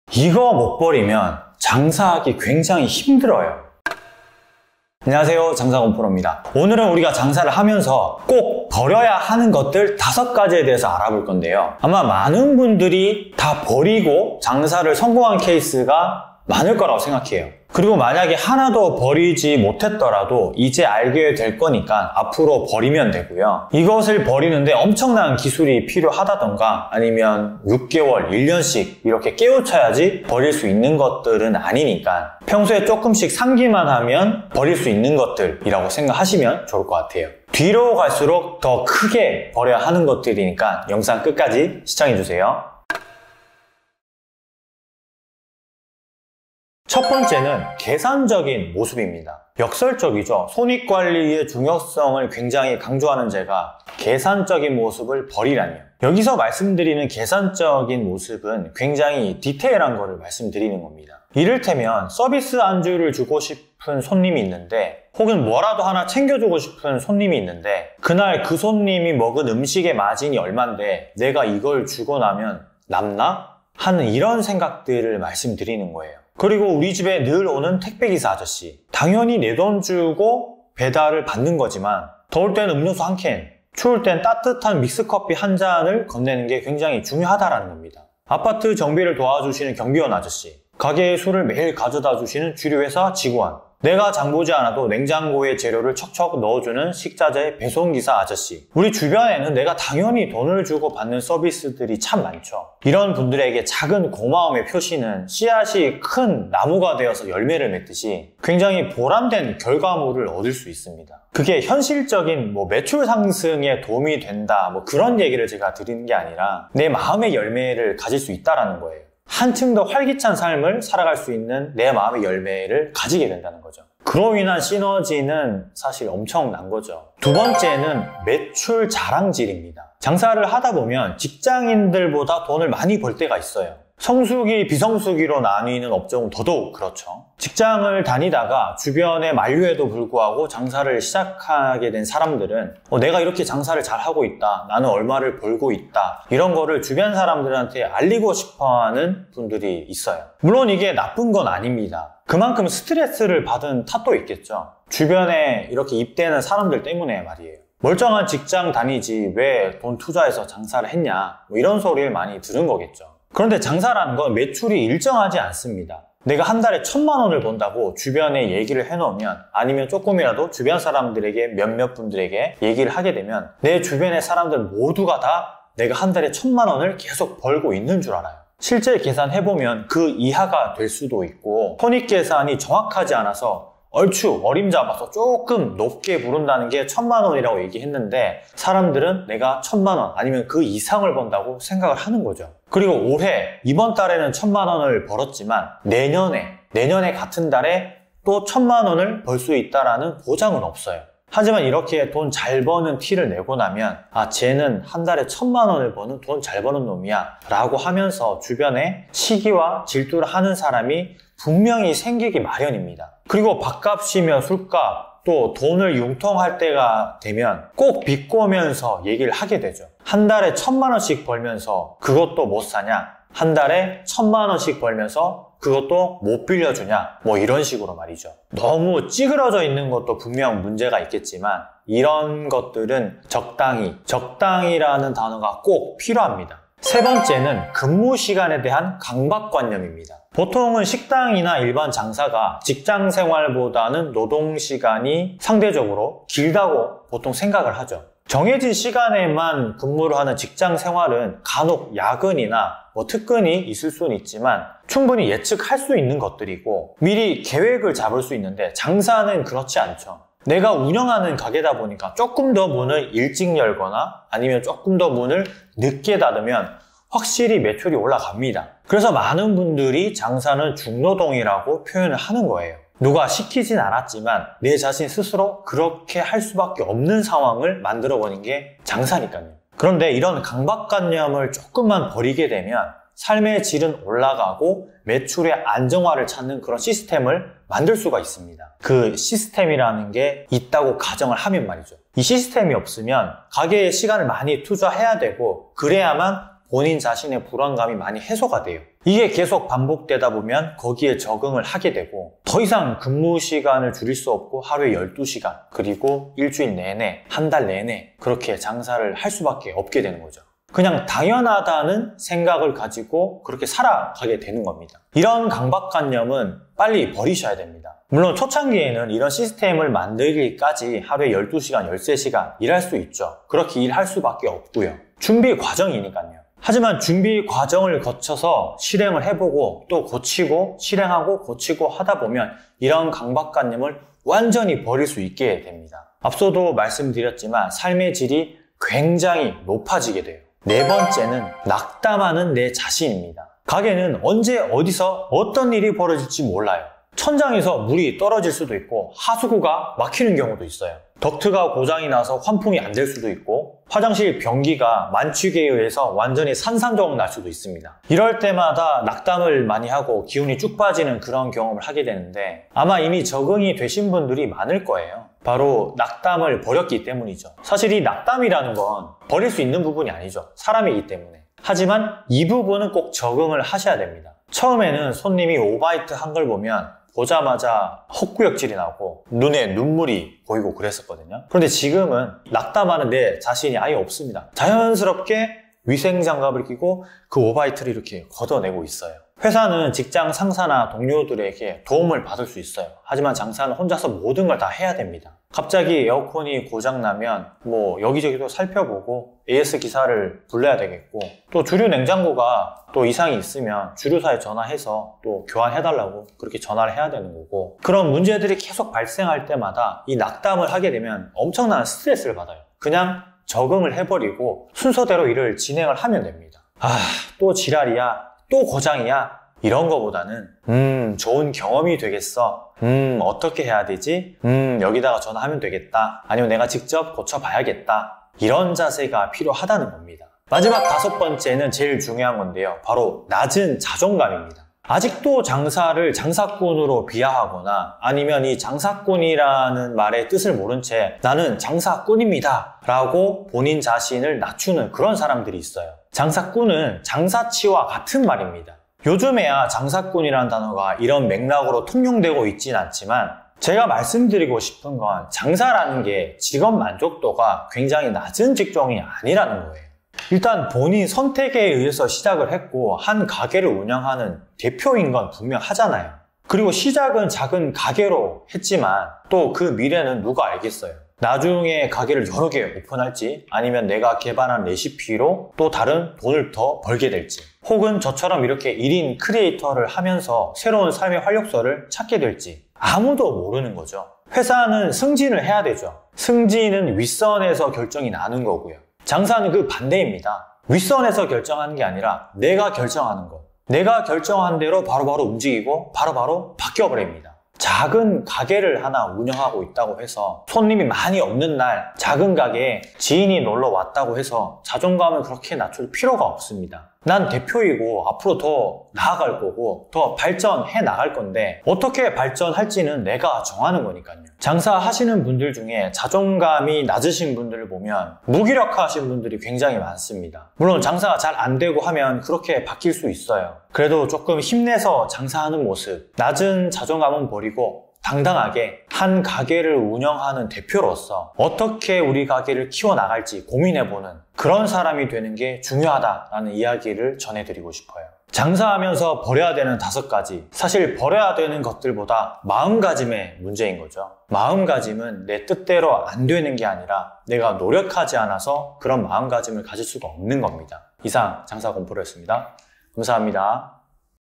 창업 전문가는 절대 얘기 못해주는 장사 할 때 꼭 버려야 할 5가지(비전문가)
전문가분들이 이야기 하는 건 조금은 어렵기도 하고,

그냥 비전문가인 제가 봤을때 이런걸 버리면 좀 장사 할 때 도움이 되겠다

싶은걸 나열해 봤어요.

뜬금포도 있겠지만 실제 느낀것들이니까 도움이 되었으면 해요.

장사 권프로